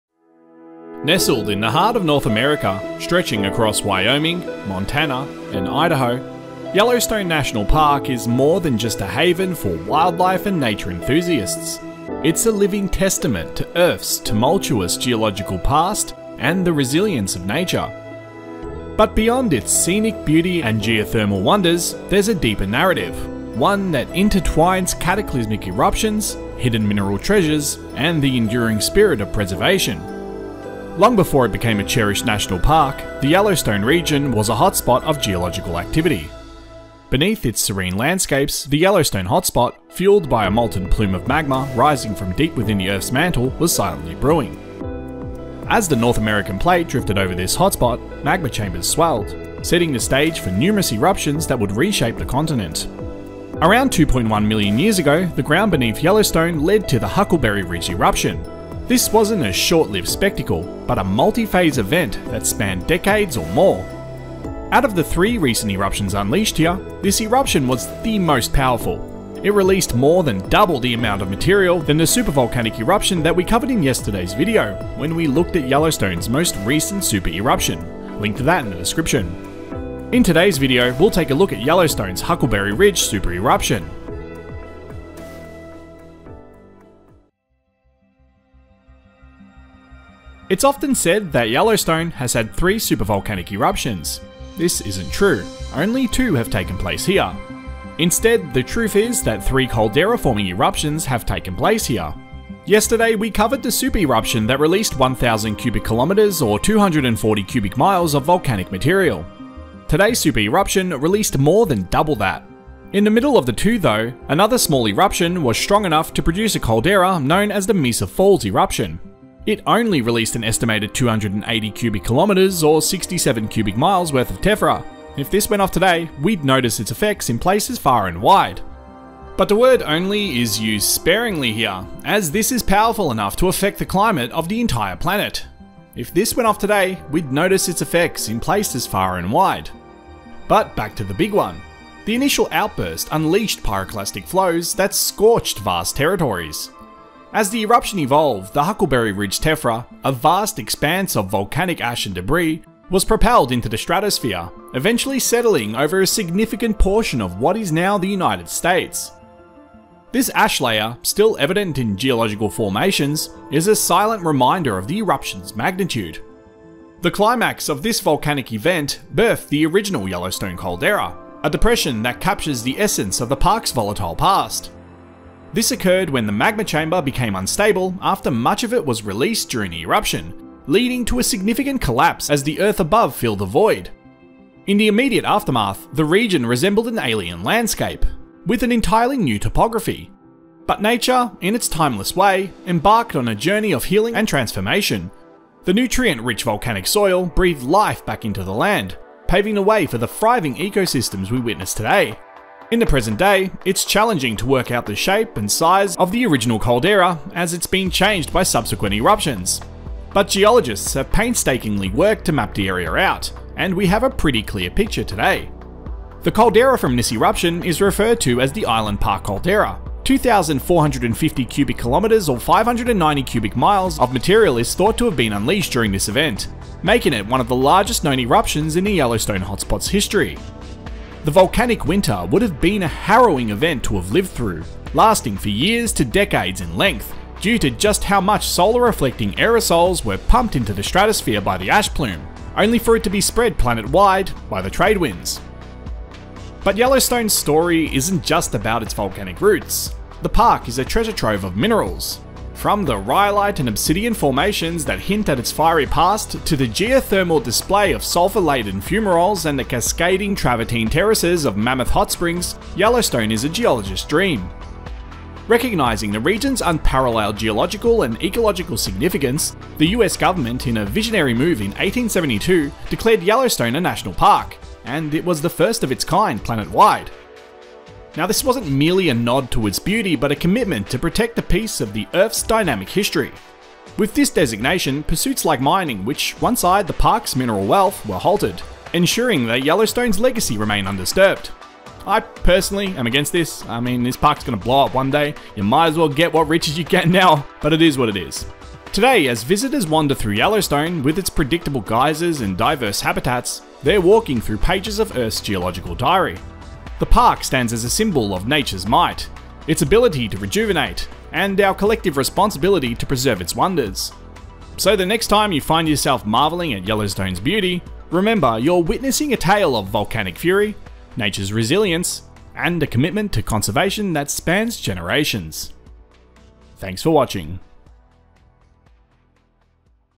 Yellowstone's Strongest Super Volcanic Eruption
Dive with us into the depths of Earth's geological diary as we explore the mysteries of Yellowstone's second and most powerful super volcanic eruption.

Yellowstone is perhaps the most well known super volcano in the world. And in this video we cover the second most recent supervolcanic eruption that worked to mold the landscape into the shape we see today.

Witness the delicate balance of preservation and evolution. If you're passionate about our planet's rich history, diverse ecosystems, and the stories they hold, you're in the right place.

Click that bell icon to be notified of when we upload new videos.